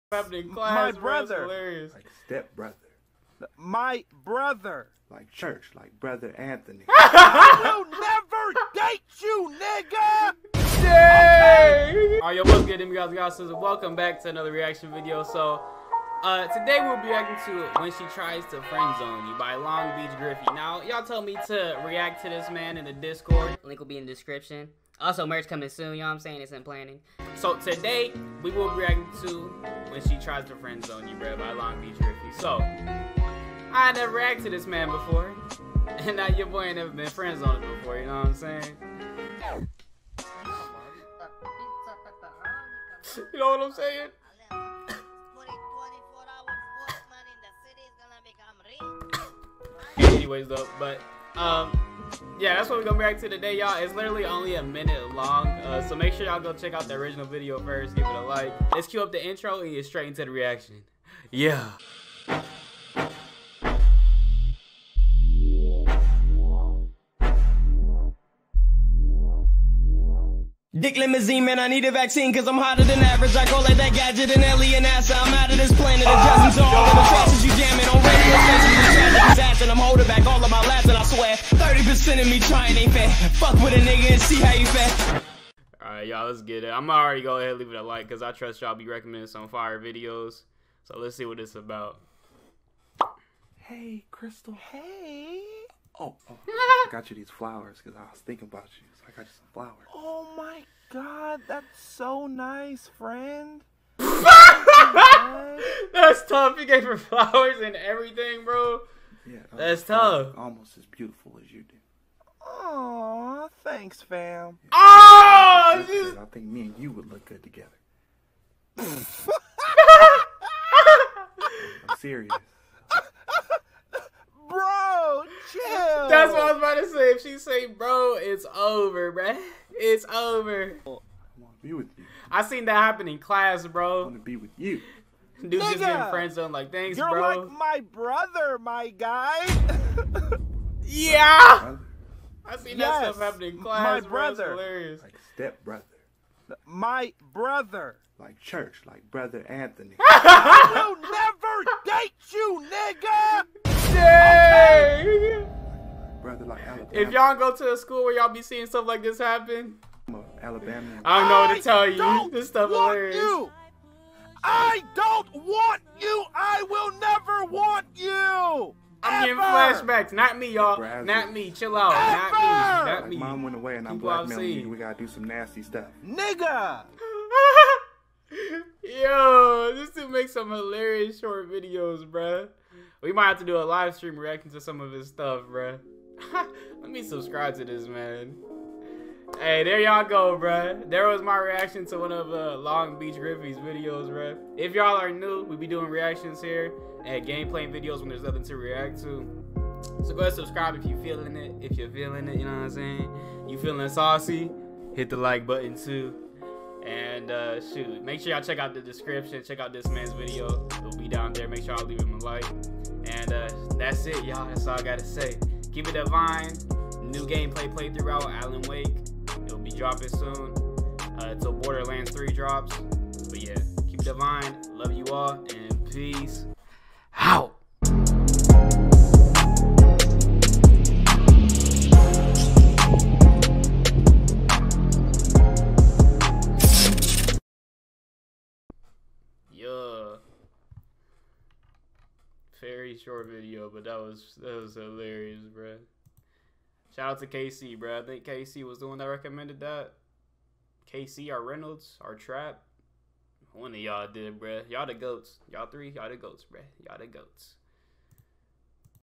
MOST SAVAGE FRIENDZONE! When she tries to friendzone you REACTION!!!
My first LongBeachGriffy reaction, lets see what these vids be like!

Connect on the socials 😉😎-

Lee’s Xbox GT: Lee The Sage

Profile Picture Artist - @kaede_draws